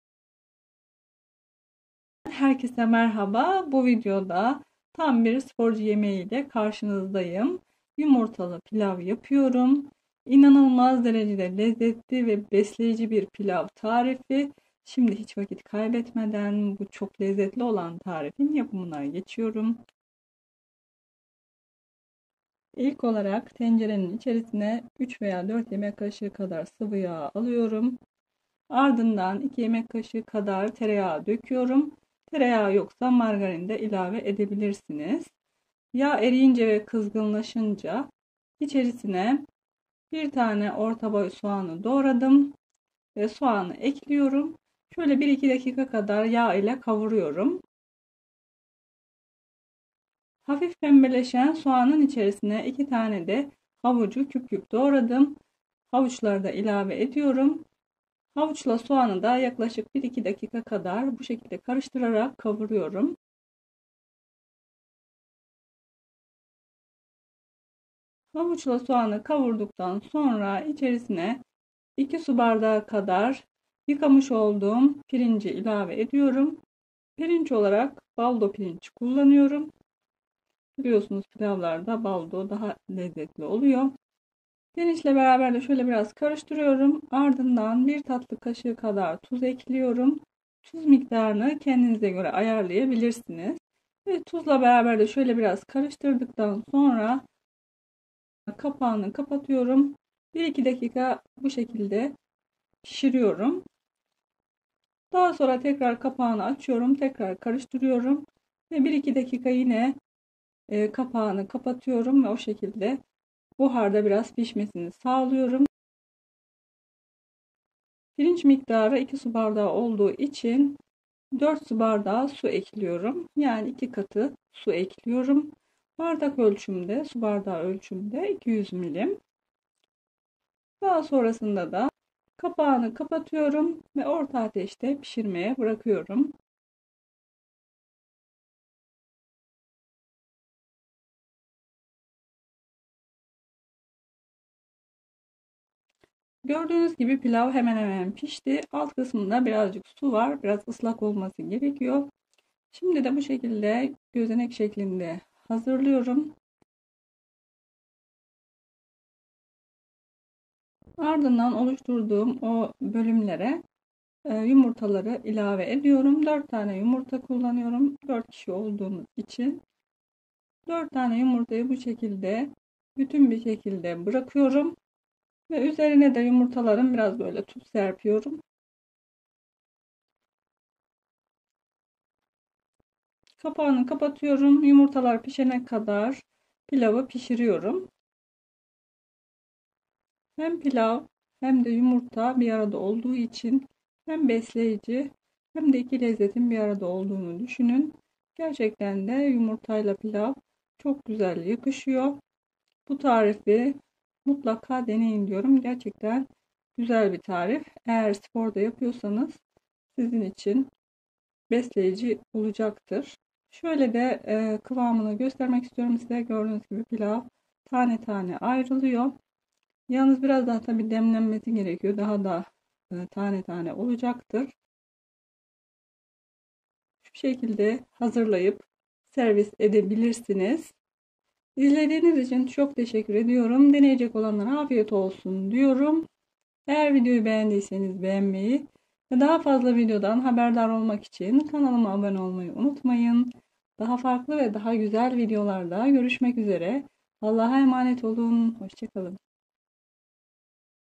BU GÜNDEN SONRA PİRİNCİ HEP BÖYLE PİŞİRMEK İSTEYECEKSİNİZ.DİKKAT BAĞIMLILIK YAPAR.LEZETLİ YEMEK
Bu tariften sonra pilavı hep böyle yapmak isteyeceksiniz .Çok lezzetli ve besleyici pilav tarifi. Tane tane yumurtalı pilav ,kolay yemek, farklı yemek, yemek tarifleri ,yemek çeşitleri, yemek, tarif

Malzemeler:
2 su bardağı pirinç
1 tane orta boy soğan
2 tane havuç
1 tatlı kaşığı tuz
4 su bardağı su
4 tane yumurta fazla veya az olabilir
4 yemek kaşığı sıvı yağ
2 yemek kaşığı tereyağı veya margarin

Afiyet olsun   :)

HAFTADA 2 GÜN NEFİS TARİFLERLE KARŞINIZDAYIM.💛
EĞER ZİL İŞARETİNİDE AKTİFLEŞTİRİRSENİZ VİDEO YAYINLANDIĞINDA BİLDİRİM ALIRSINIZ.. 💛
💛SEVGİLER💛